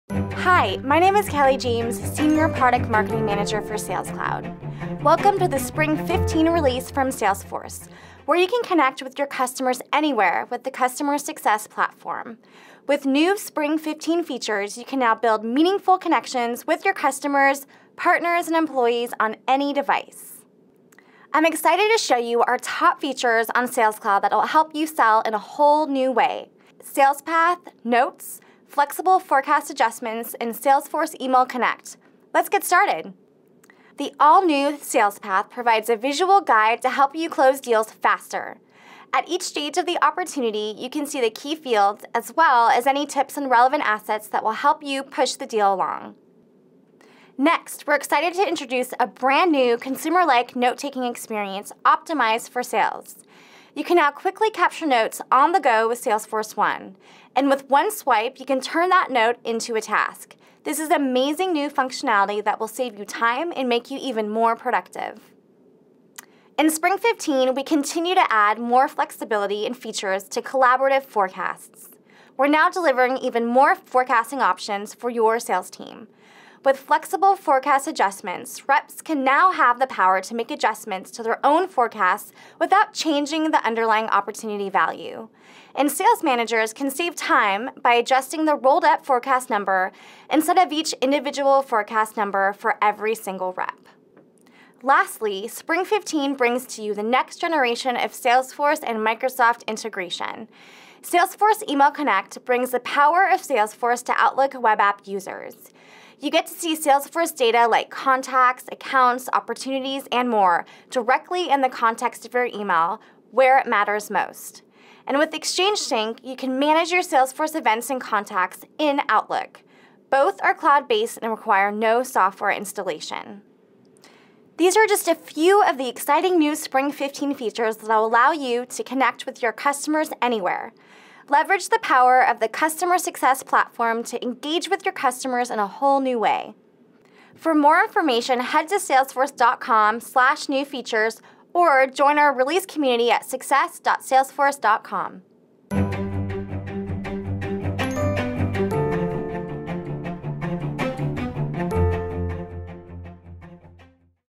Sales Cloud – Spring ’15 New Features
Sell in a whole new way with the world’s #1 CRM sales app – the Salesforce Sales Cloud. This video provides an overview of the Spring ’15 release.